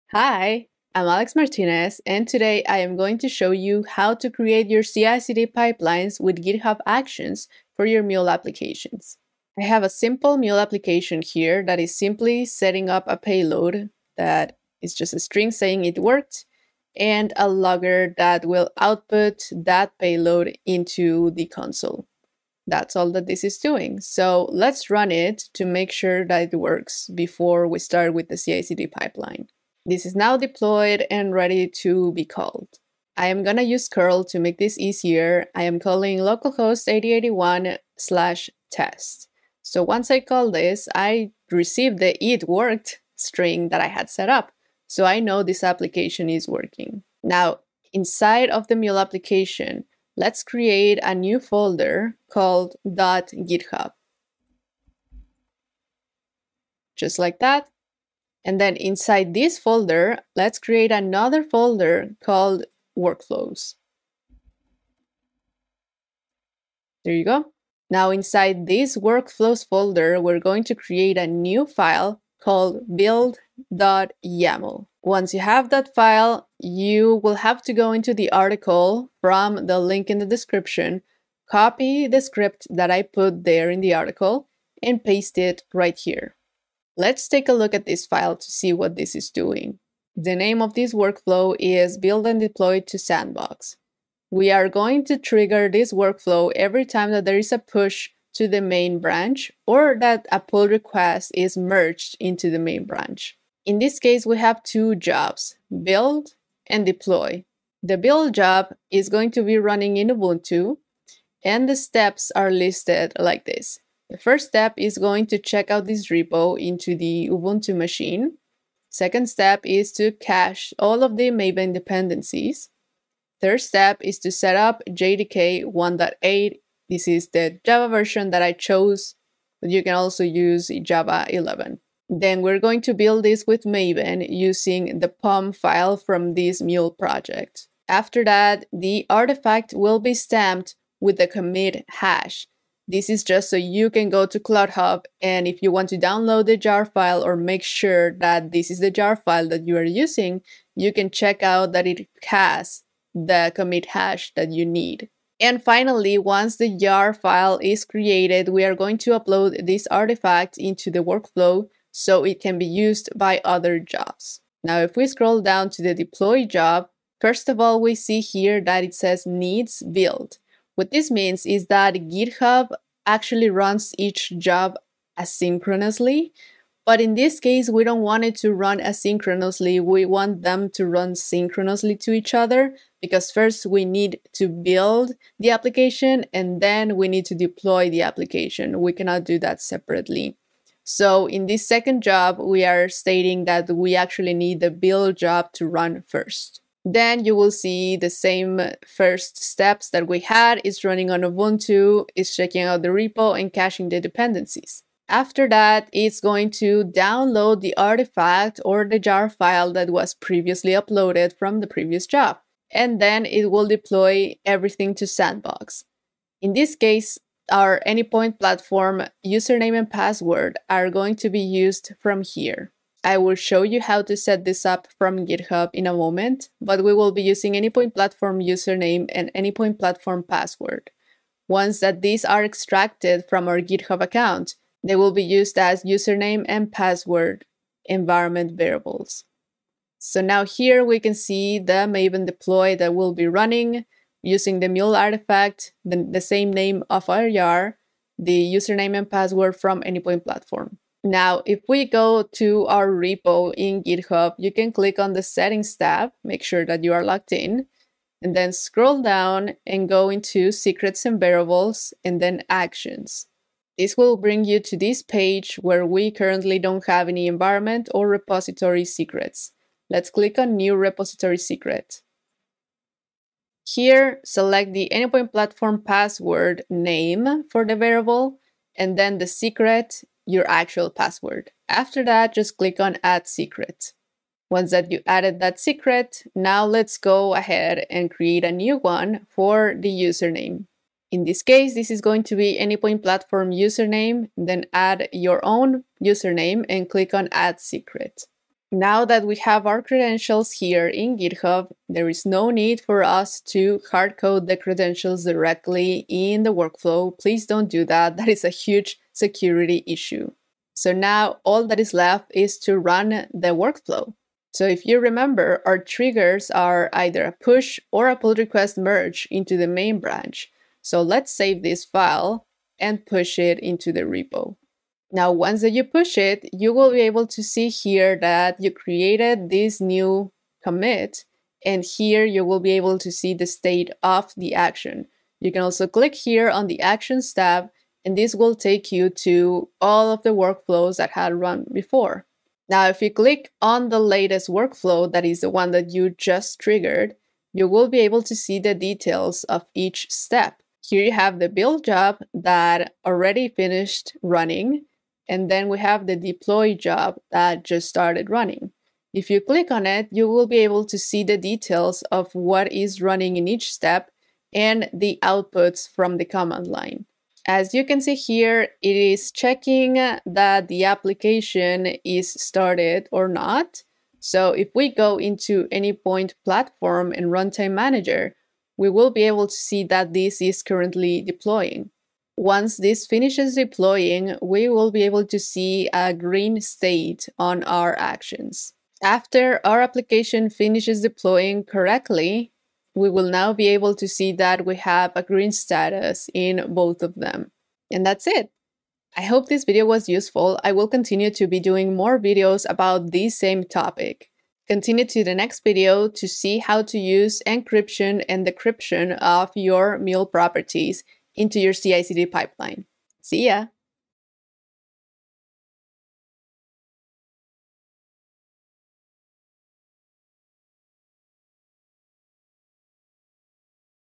Part 1: How to set up a CI/CD pipeline to deploy your MuleSoft apps to CloudHub using GitHub Actions
Creating CI/CD (Continuous Integration, Continuous Delivery) pipelines for your code has become a standard practice. Instead of worrying about deployments and keeping your environments up to date, you can set up this automated pipeline to do the deployments for you. You can focus on developing your code and let the pipeline take care of the rest.

In this video, we’ll learn how to create a simple CI/CD pipeline to deploy Mule applications from GitHub to CloudHub using GitHub Actions.